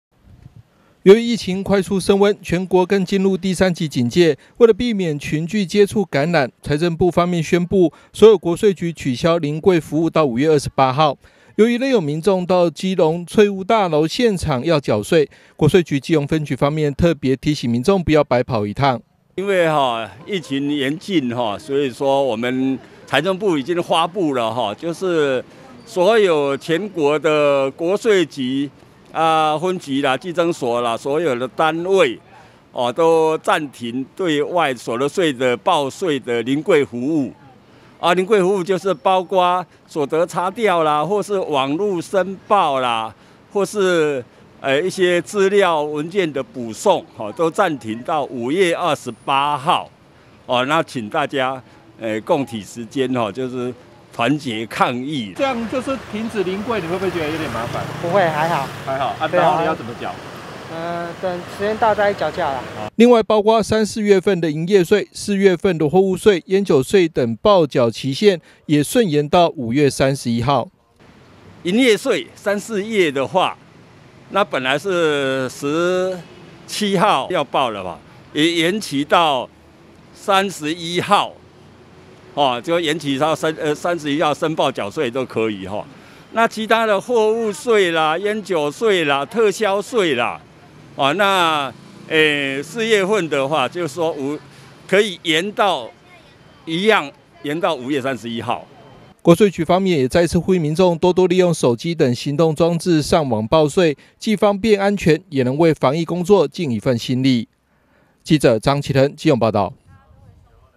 基隆 疫情|線上 平台|取消 防疫|確診 本土|稅 期限|報稅 季節|中嘉新聞 2021|全國疫情進入第三級 國稅局暫取消臨櫃繳稅 營業稅等延至5月底
避群聚風險國稅局櫃台5月28日前取消臨櫃服務民眾別白跑一趟

（中嘉吉隆新聞記者張啟騰/基隆報導）五月進入報稅季節，不過受到疫情升溫全國進入第三級警戒影響，財政部方面也宣布所有國稅局取消臨櫃服務到5/28。至於營業稅貨物稅菸酒稅等報繳期限也延至5/31，提醒報稅民眾不要白跑一趟。